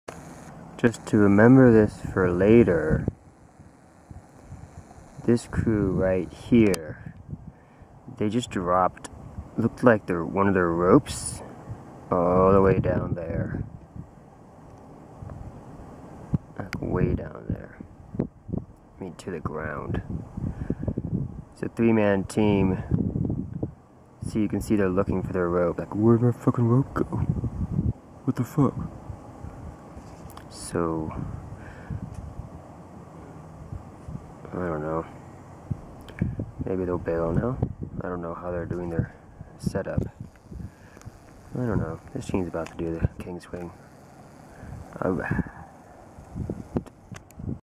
Nose Team that Bailed 9/18/13.
Nose Team that Bailed 9/18/13. A team after they dropped their ropes on the nose and then had to bail